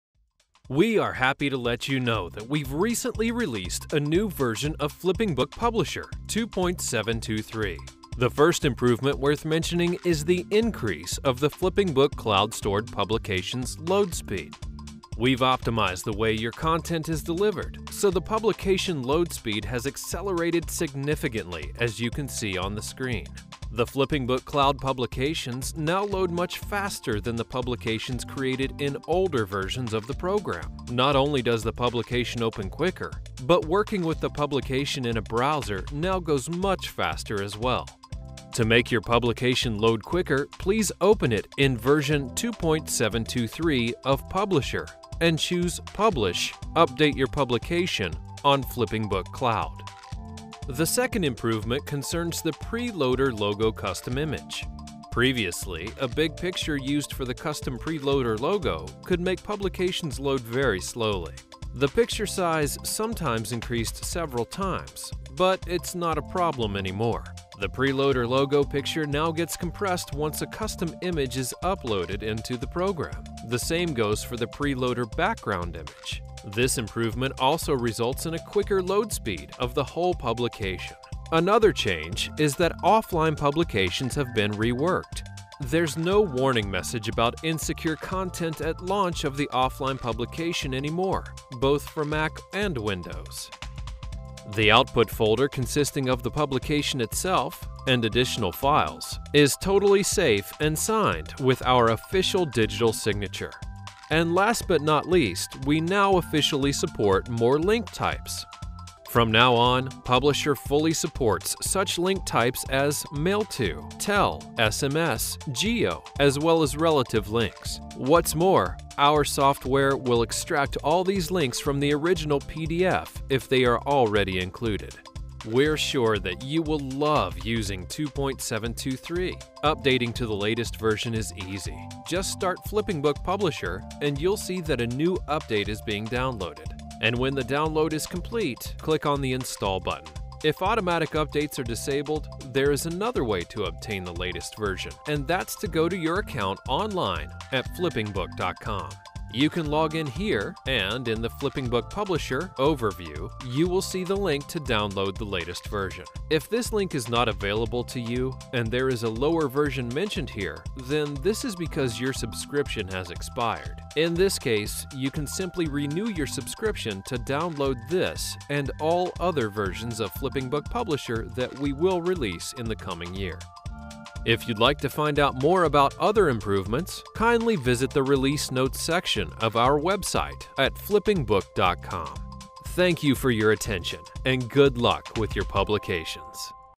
FlippingBook Publisher update: What's new in the version 2.7.23
In the new update of FlippingBook Publisher, we improved preloaders and publications' load speed, as well as re-worked access to offline publications. Take a look!

Read the full list of our release notes for version 2.7.23